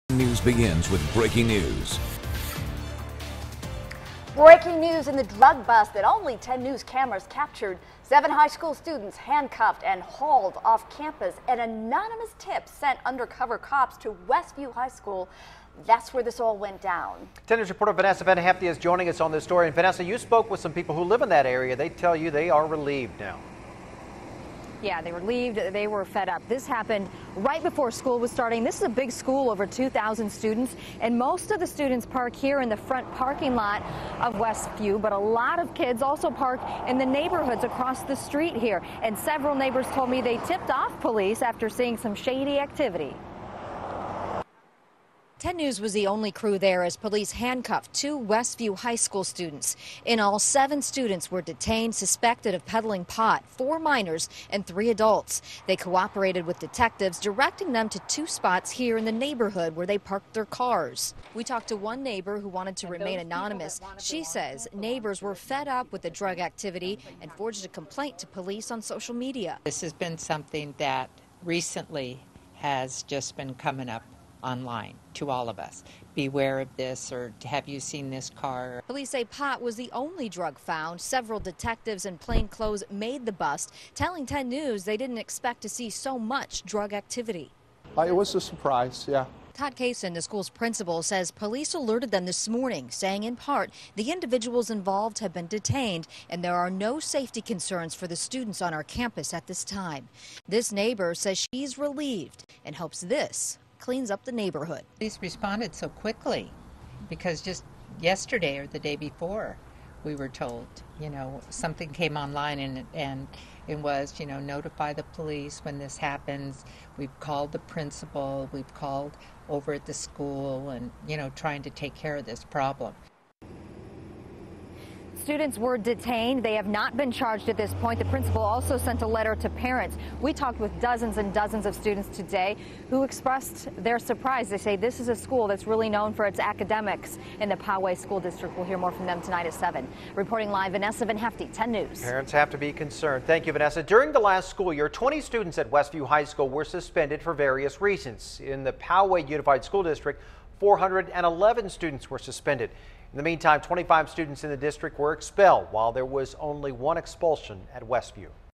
7 Westview High students detained in pot bust
Anonymous tip sent undercover cops to campus.